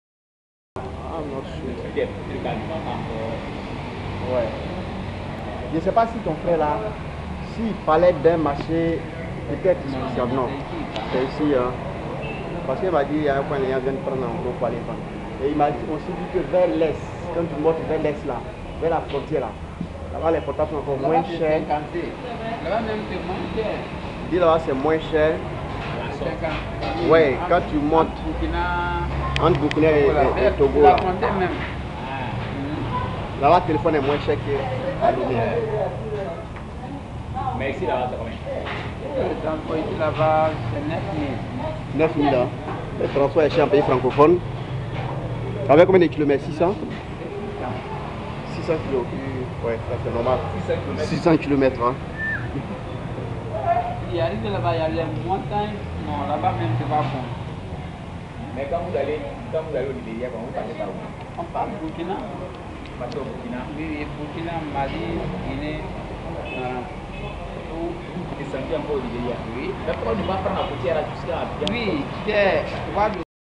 AFRICA - LOME CAPITALE DU TOGO EN 2012 - SUITE
Kapohoney à Lomé au Togo en Février 2012, avec mon oncle Paulson KOUE, accompagnés de notre hôte.